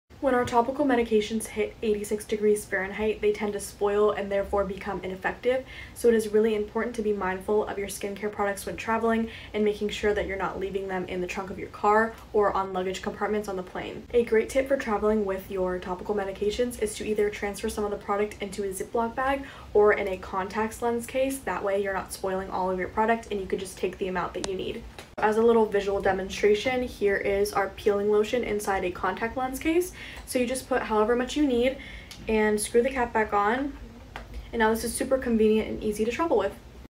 Keeping your Products Cool for the Summer
If your skin products heat up, they will spoil and no longer work. Helpful tips on how to travel with your medication.